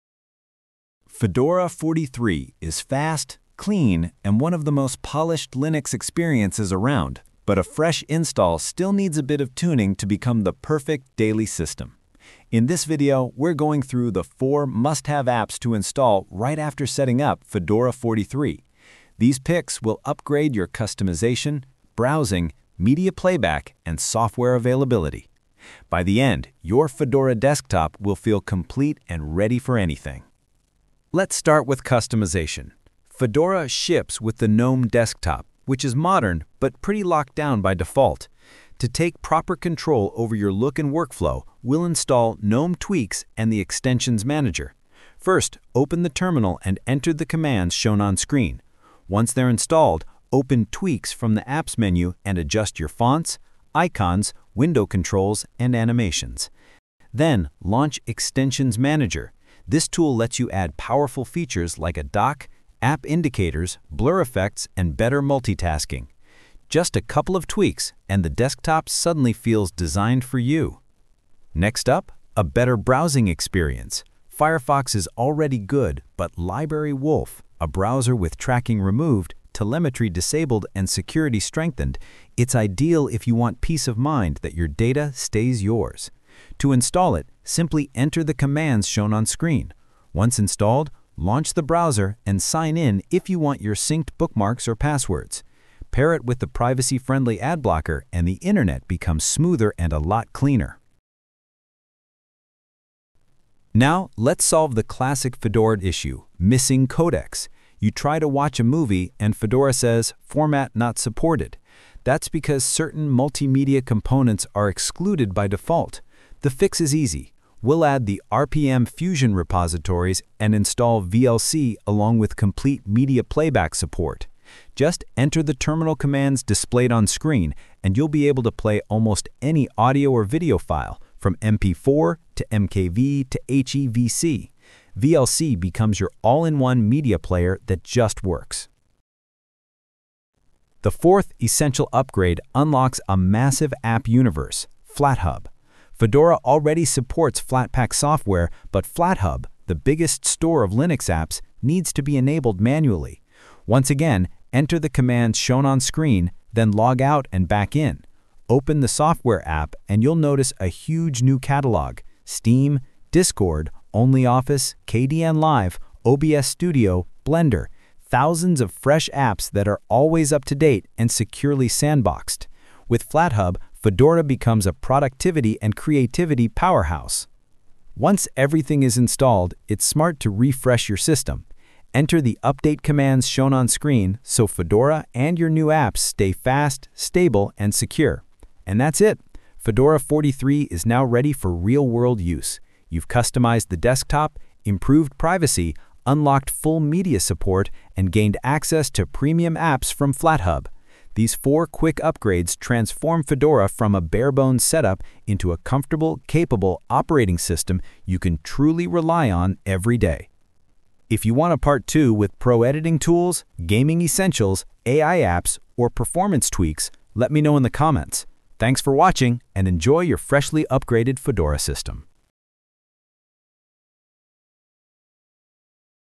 Fedora 43 Workstation — 4 Must Things to Do After Install (Quick Setup Guide 2025)
Just installed Fedora 43? Do these 4 must things to unlock the real power of your system. This quick 3-minute setup guide gives you customization tools, a privacy-focused browser, full media support, and access to thousands of apps — all with simple commands shown on screen.

What you get in this video:
• GNOME Tweaks & Extensions for custom desktop control
• LibreWolf browser for privacy and security
• VLC + codecs with RPM Fusion for full media playback
• Flathub for the latest software and games

Commands included in pinned comment.
Perfect for Fedora beginners or anyone rebuilding their Linux setup.

Chapters:
00:00 Intro
00:20 Customization Tools
01:05 Privacy Browser
01:50 Media Support
02:40 App Store Access
03:20 Final Tips